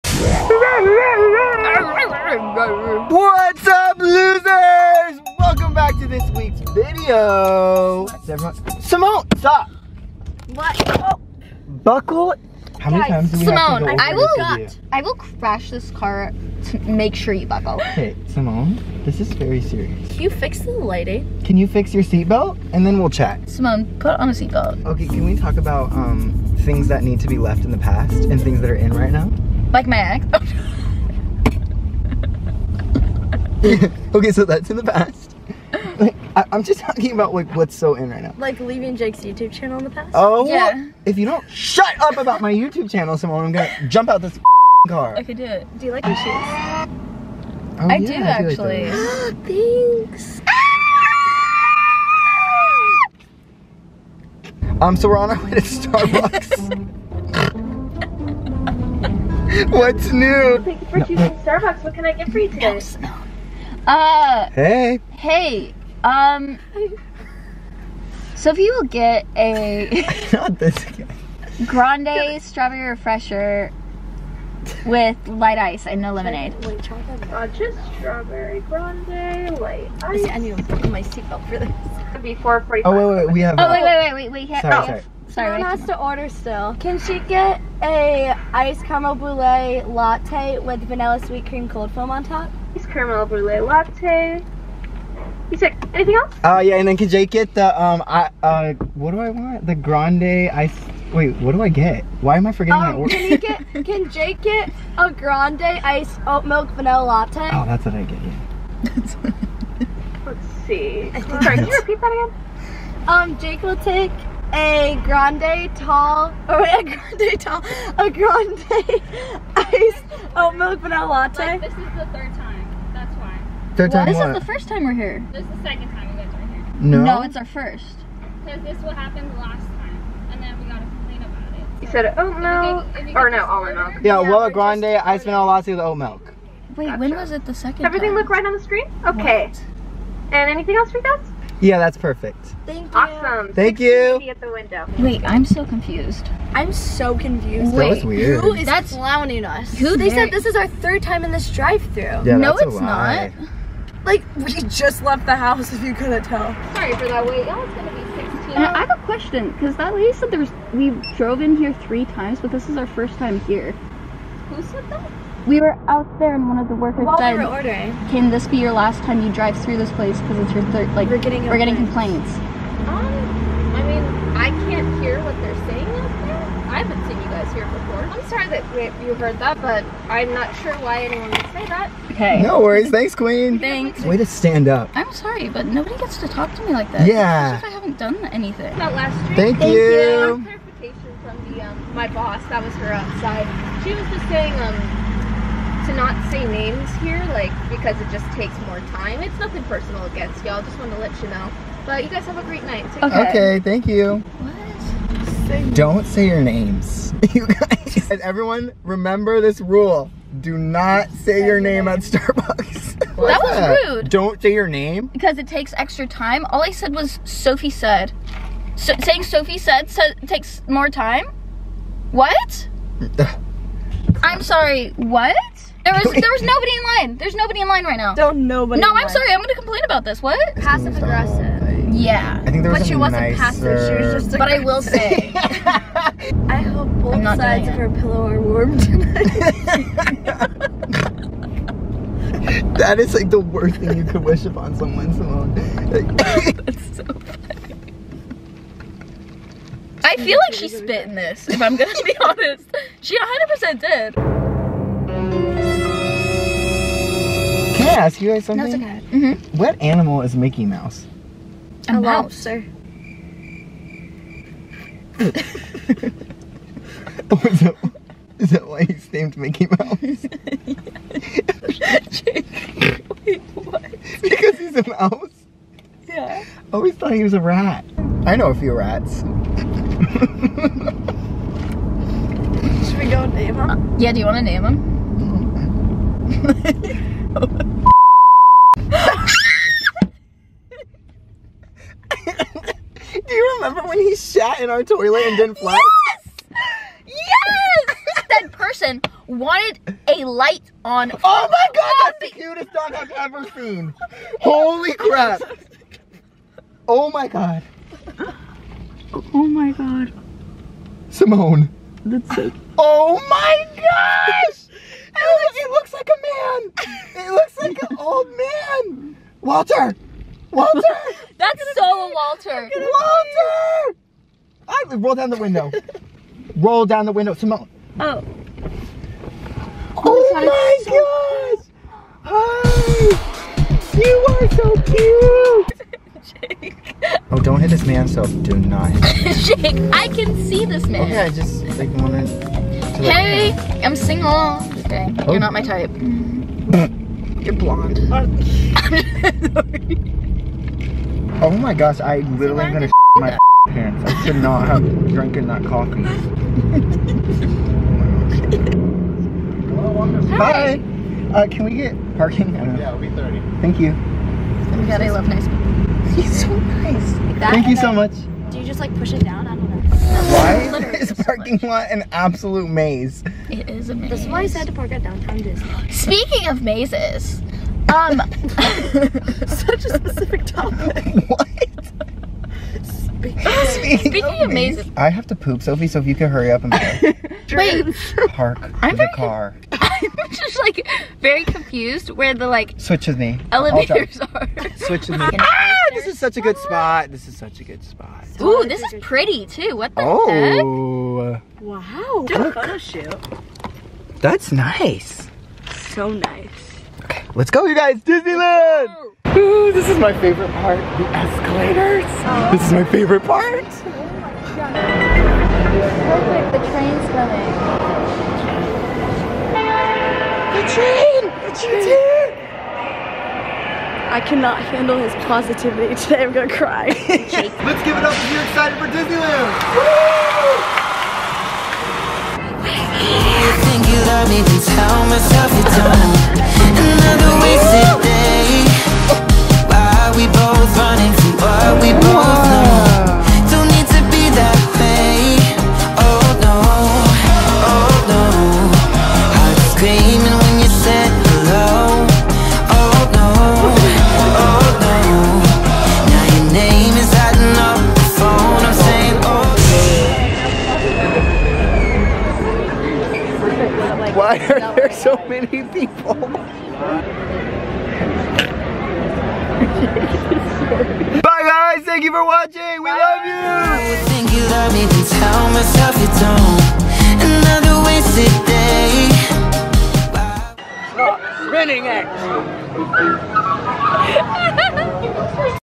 well that's one way to PISS ME OFF!
On my weekly coffee run with @SymonneHarrison and @sofiedossi  things didn't quite go as planned!! Some bad attitudes ruined our vibes! HOWEVER, we couldn't let that bring down the amazing time we were having, so we decided to last minute go to DISNEYLAND!